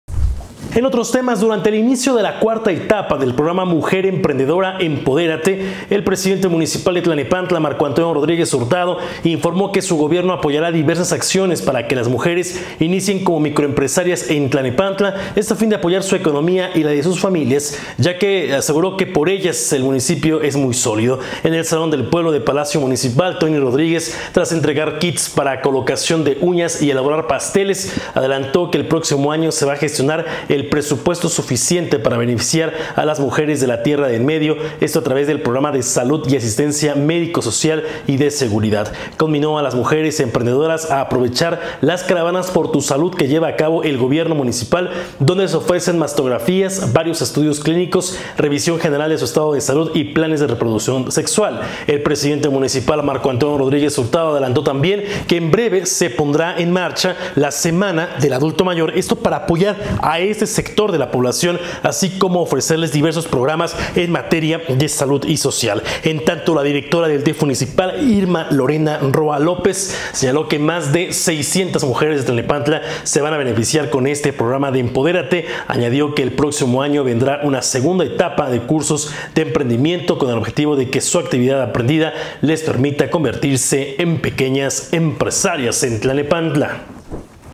Gobierno de Tlalnepantla apoya a mujeres para convertirse en pequeñas empresarias
Durante el inicio de la cuarta etapa del "Programa Mujer Emprendedora, Empodérate", el Alcalde Marco Antonio Rodríguez Hurtado informó que su gobierno apoyará diversas acciones para que las mujeres inicien como microempresarias en Tlalnepantla, a fin de apoyar su economía y la de sus familias, ya que "por ustedes nuestro municipio es muy sólido".

En el Salón del Pueblo del Palacio Municipal, Tony Rodríguez tras entregar los kits para colocación de uñas y elaborar pasteles, adelantó que el próximo año se gestionará el presupuesto suficiente para beneficiar a las mujeres de "La Tierra de En Medio", a través de programas de salud y asistencia médica, sociales y de seguridad.

Conminó a las mujeres emprendedoras a aprovechar las caravanas por tu salud que lleva a cabo el gobierno municipal, donde se ofrecen mastografías, varios estudios clínicos, revisión general de su estado de salud y planes de reproducción sexual

El Presidente Municipal, Marco Antonio Rodríguez adelantó también que en breve se pondrá en marcha la Semana del Adulto Mayor para apoyar a este sector de la población, así como ofrecerles diversos programas en materia de salud y social.

En tanto, la Directora del DIF Municipal, Irma Lorena Roa López, señaló que más de 600 mujeres de Tlalnepantla se van a beneficiar con este programa de Empodérate. Añadió que el próximo año vendrá una segunda etapa de cursos de emprendimiento, con el objetivo de que su actividad aprendida les permita convertirse en pequeñas empresarias.